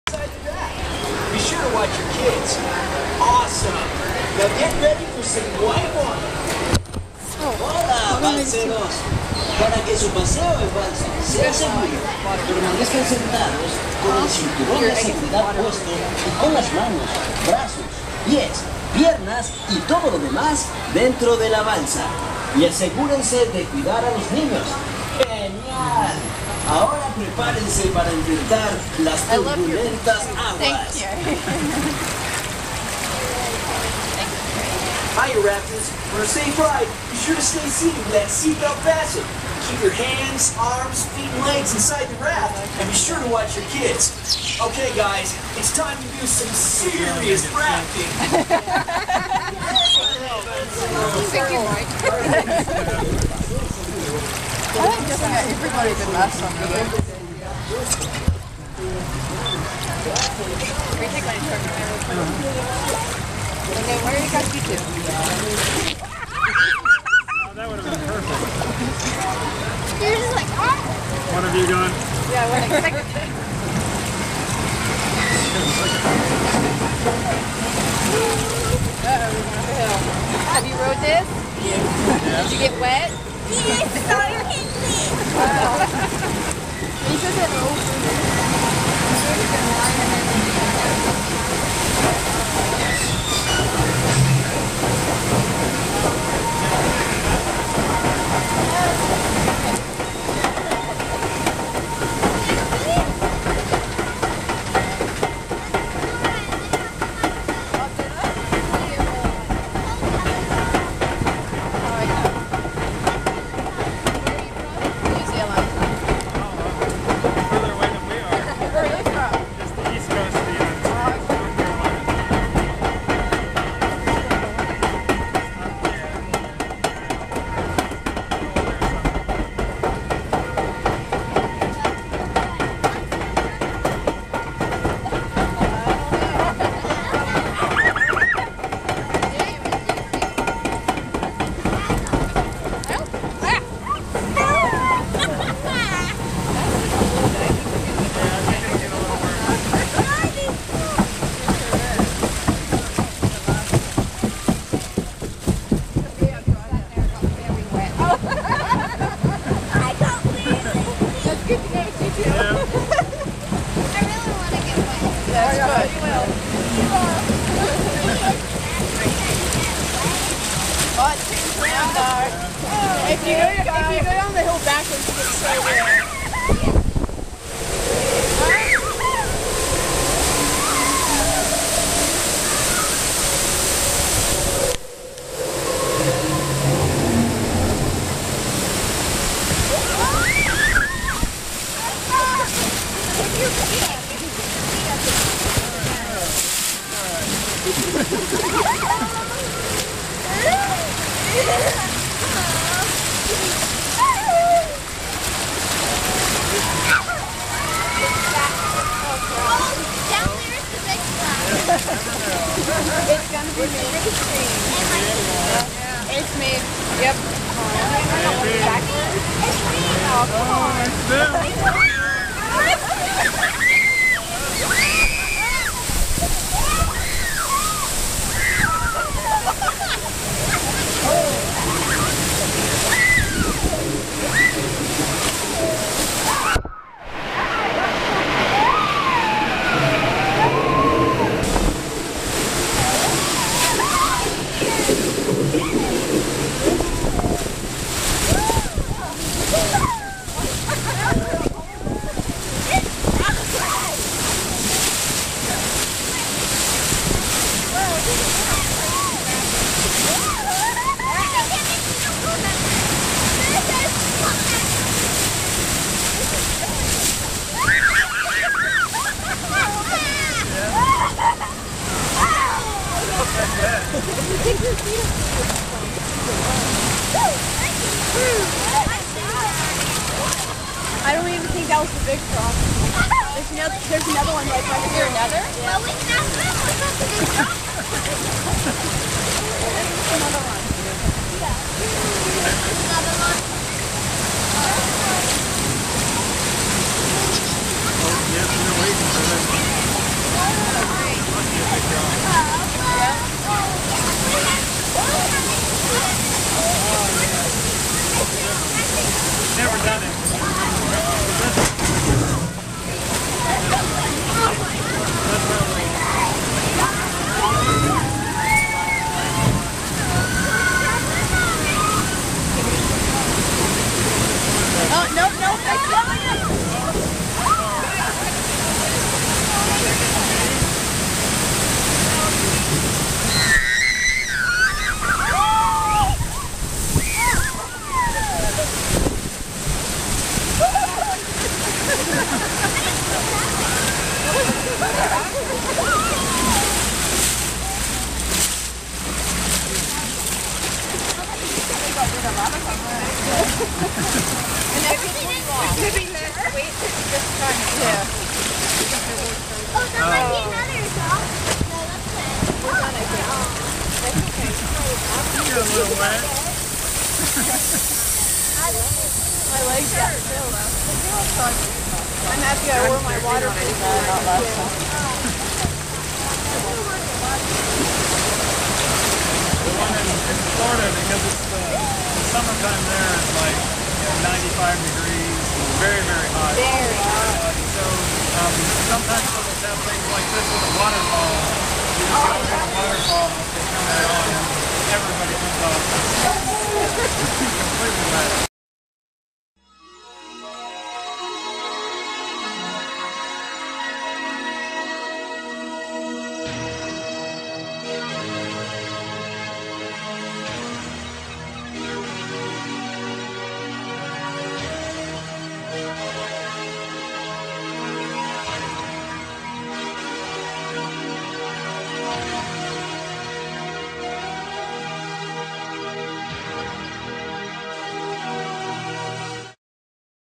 Grizzly River Run On-Ride POV 2012
An On-Ride POV of Grizzly River Run in Disney's California Adventure Park. Enjoy!